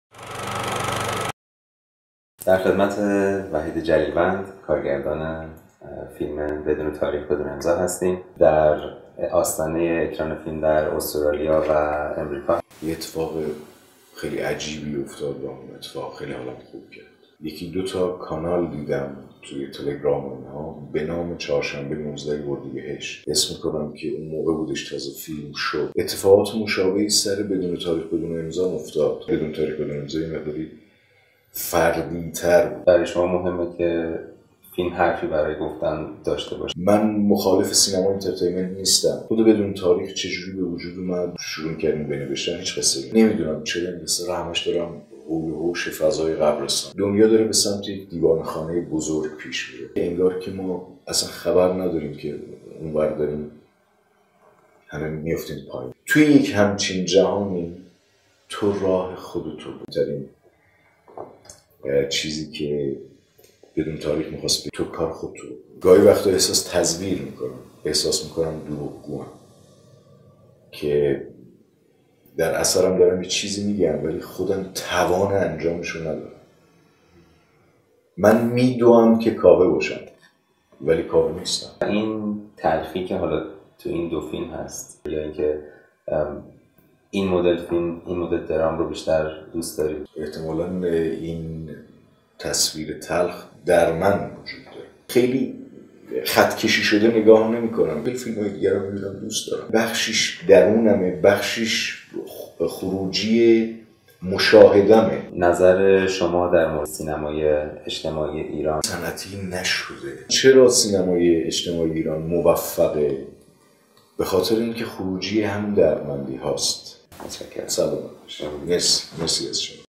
Interview with Vahid Jalilvand - Director of NO Date, NO Signature - (In Farsi) By Daricheh Cinema
Daricheh Cinema’s Exclusive Interview (summary: 2min) ; Armin Miladi in conversation with Vahid Jalilvand, Director of No Date No Signature (Bedun-e Tarikh, Bedun-e Emza)